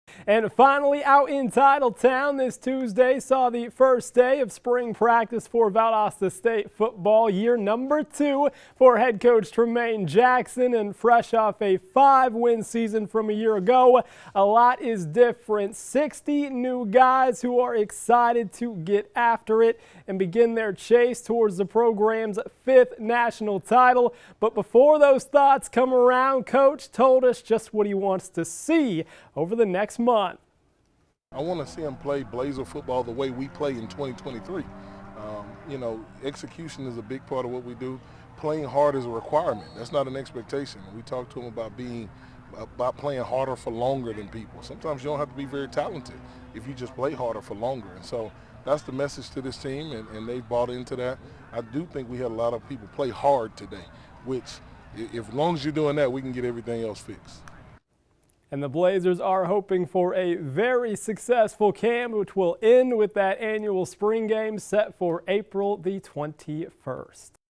Valdosta State football opens spring practice
Out in Titletown, Tuesday saw the first day of spring practice for Valdosta State football.